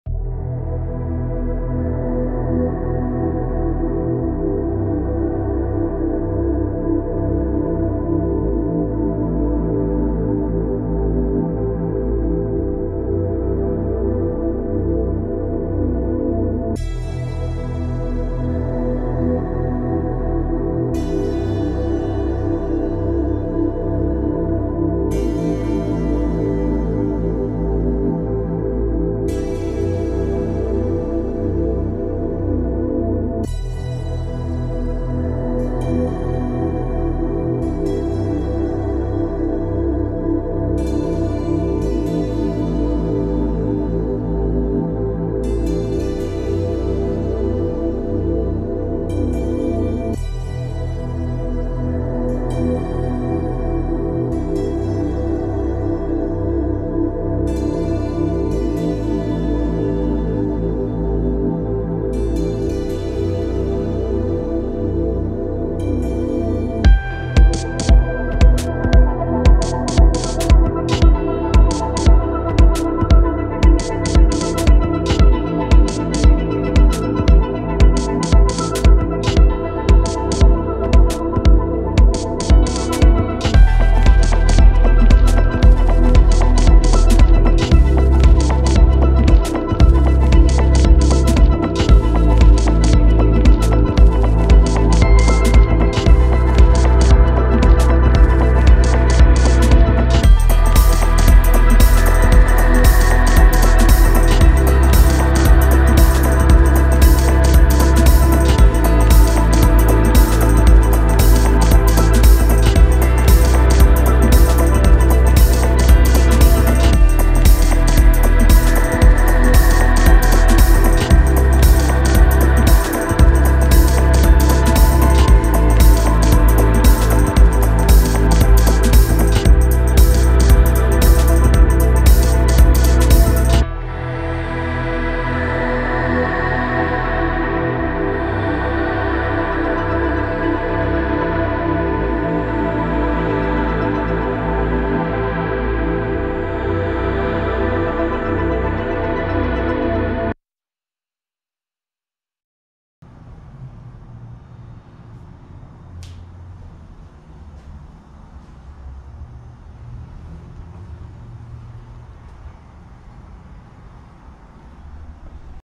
Newly renovated 2 bedroom apartment at The Croft - high end finishes!
Take a video tour of this one of a kind, newly renovated 2 bedroom apartment home with high end finishes, stainless appliances, and W/D!